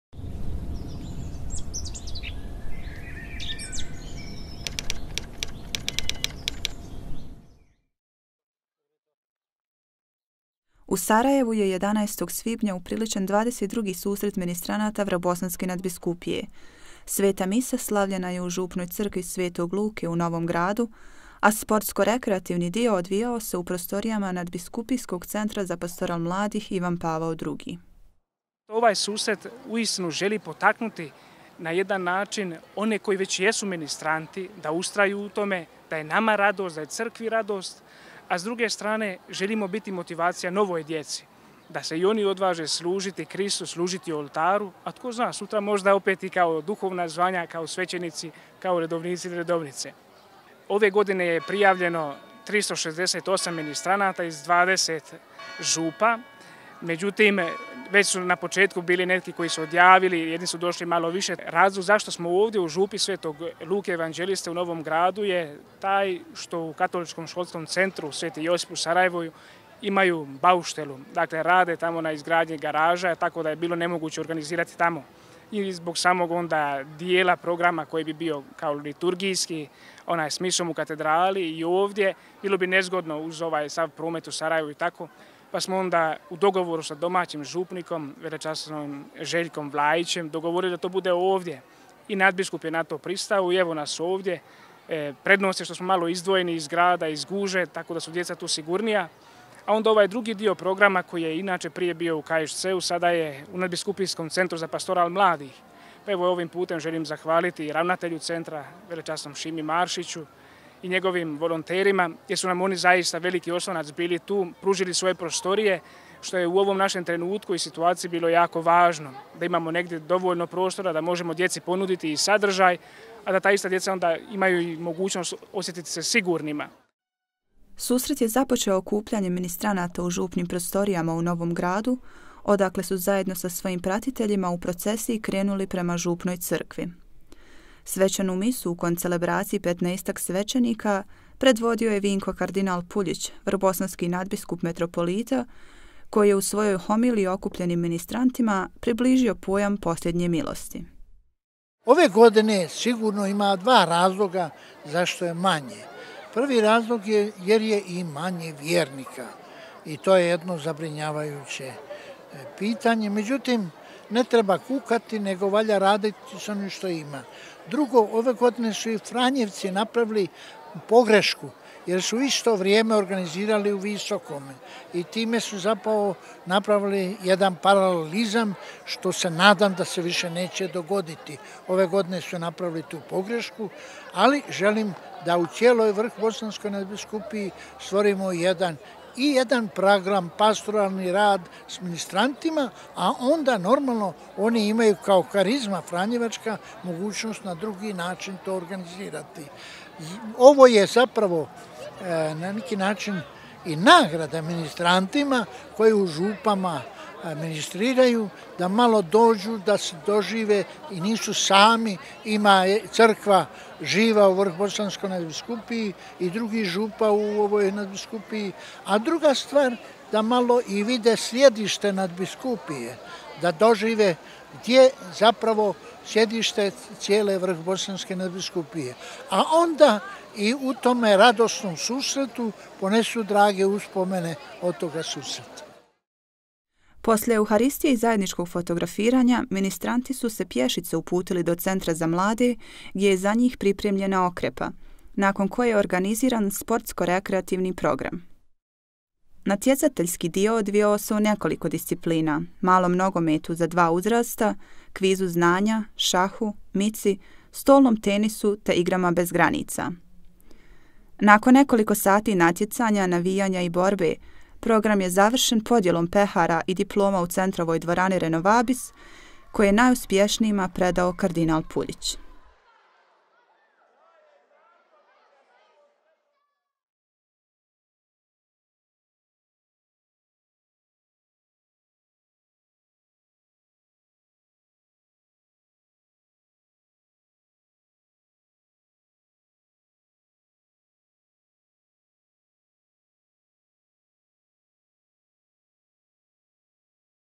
XXII. susret ministranata Vrhbosanske nadbiskupije
U Sarajevu je 11. svibnja upriličen XXII. susret ministranata Vrhbosanske nadbiskupije. Sveta misa slavljena je u župnoj crkvi Sv. Luke u Novom Gradu, a sportsko-rekreativni dio odvijao se u prostorijama Nadbiskupijskog centra za pastoral mladih Ivan Pavao II.